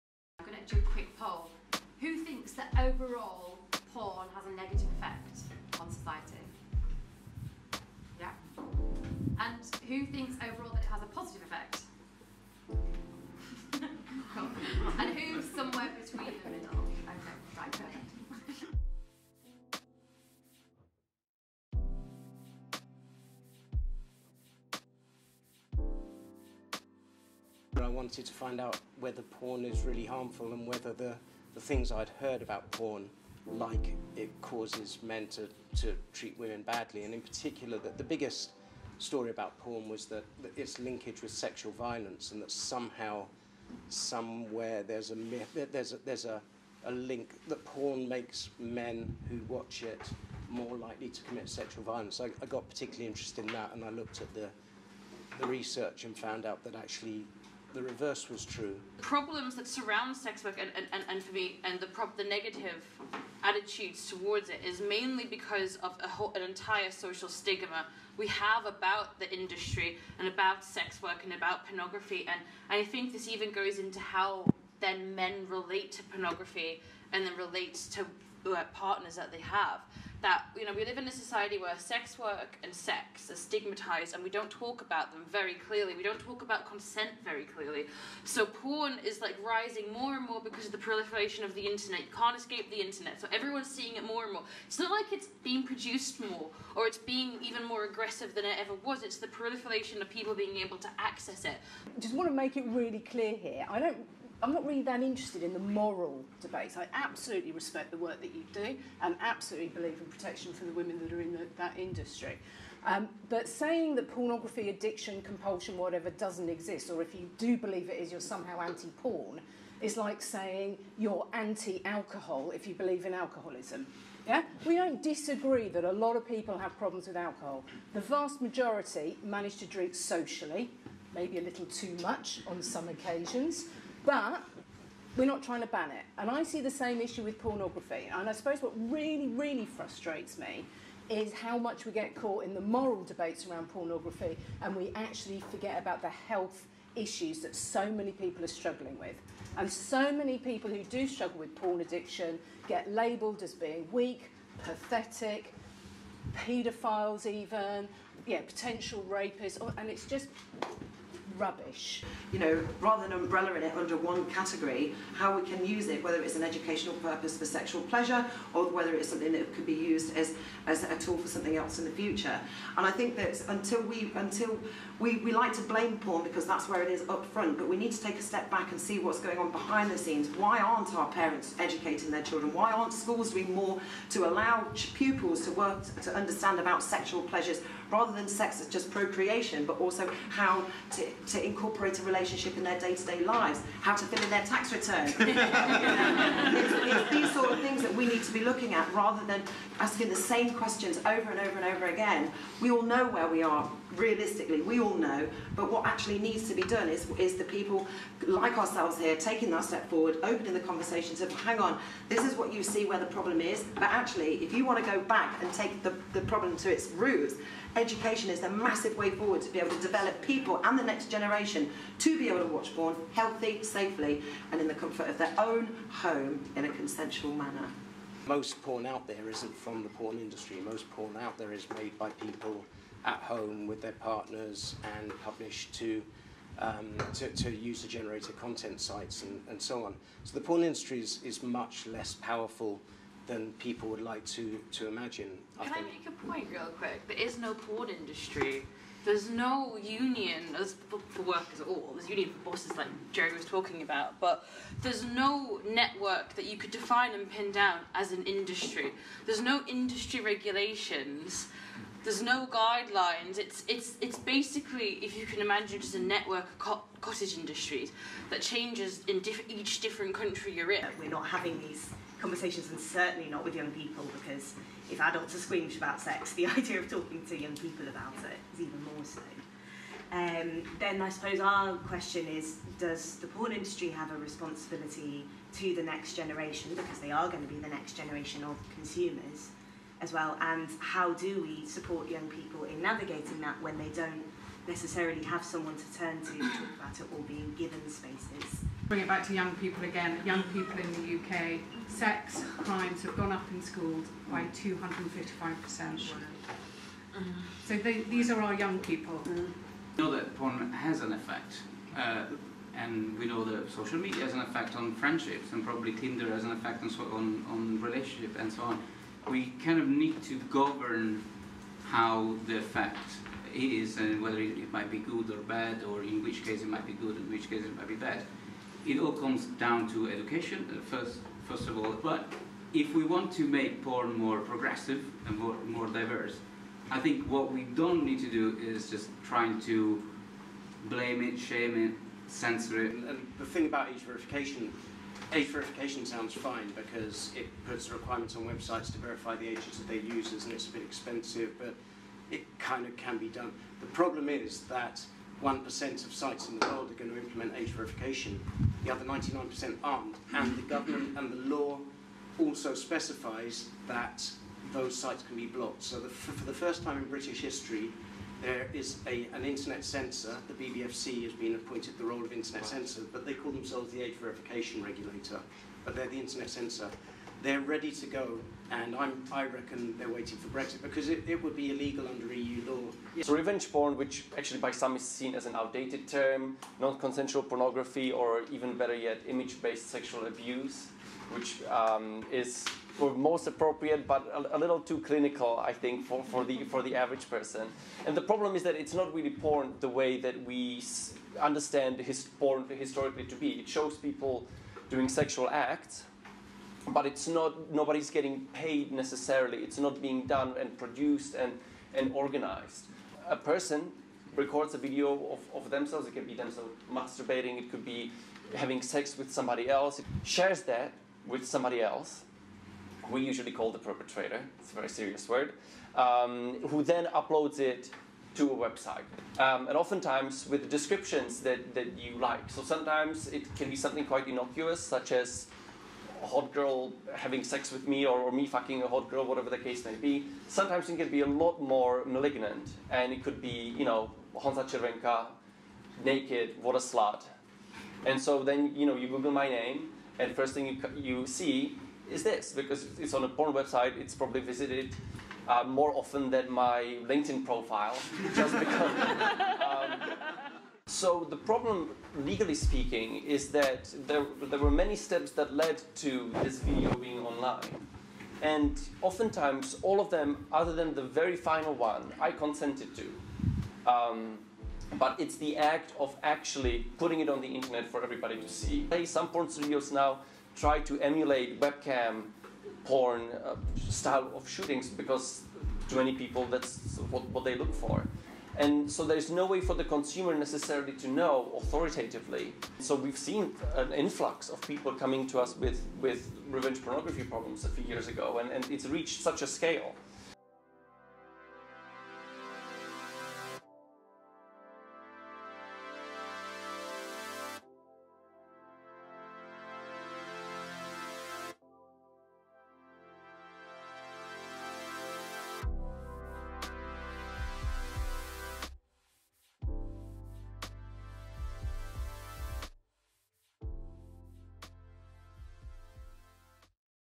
Tortoise ThinkIn – Life in the porn age
The worldwide porn industry is nearly nine times more valuable than Hollywood. Every five minutes the biggest site Pornhub transmits more data than the entire contents of the New York Public library. But in the real world, with sex “gone wrong” killings, revenge porn and rape offences on the rise, how far can we hold the adult industry responsible?

Chair: Nichi Hodgson, sex and relationships broadcaster; author, The curious history of dating: from Jane Austen to Tinder

Our special guests include:

Paula Hall, sex addiction therapist, The Laurel Centre

Charlotte Rose – sexpert and consultant

Misha Mayfair, pornstar

Jerry Barnett, Founder, Sex & Censorship Campaign

Honza Cervenka, solicitor at McAllister Olivarius, focusing primarly on revenge porn cases